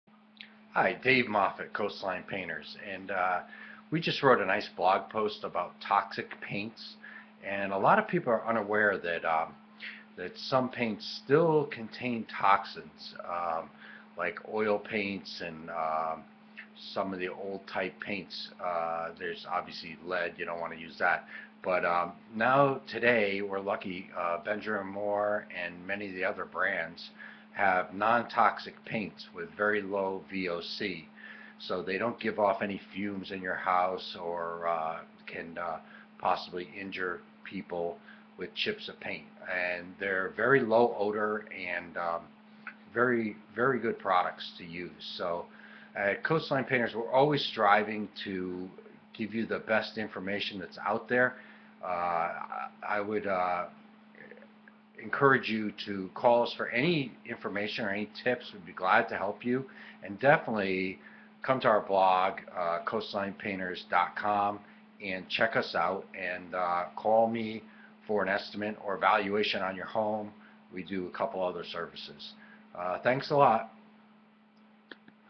painting with non-toxic paints
Why us non-toxic paints?  What are low or no voc paints?
Indoor air is three times more polluted then outside air!  A good start to reduce the level of pollution in our indoor air is the type of paint you have on your walls. Click on the link to find out more about low-voc paints and no-voic paints